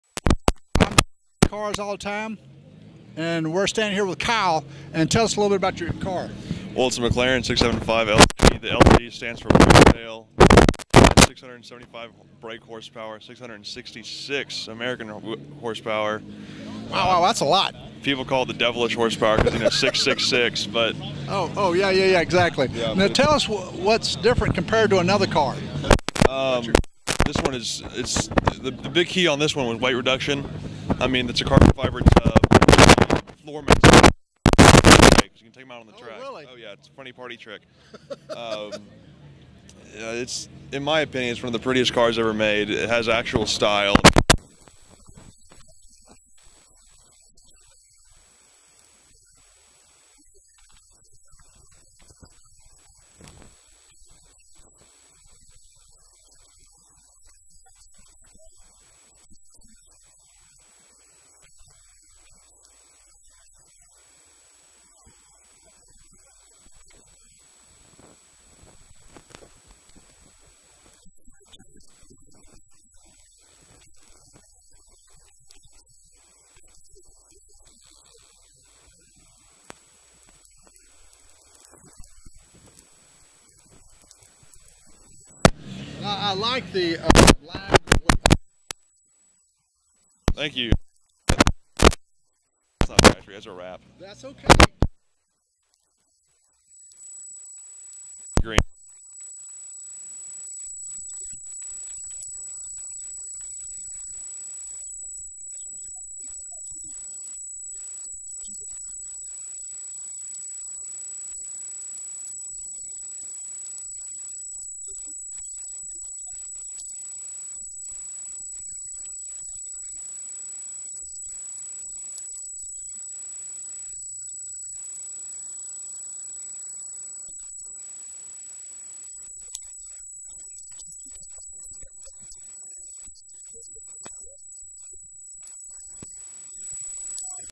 Cars All The Time search engine interview the McClaren 675LT
Cars All The Time search engine hosted by Gayland interview Kyle at the Coffee & Cars event with his the McClaren 675LT